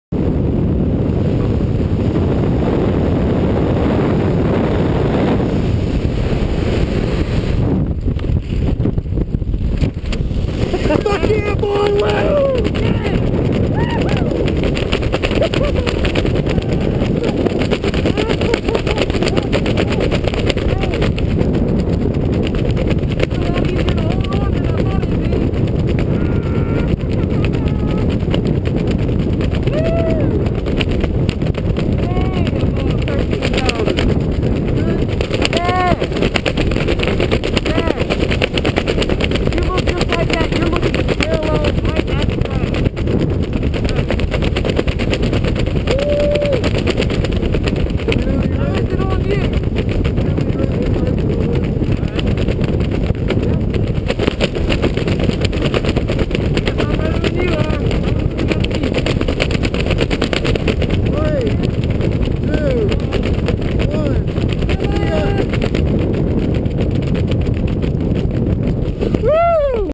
Flawless execution of a Mr. Bill.wmv
This is Michael in the red and Chad wearing the camera at Gold Coast Skydivers in Lumberton MS.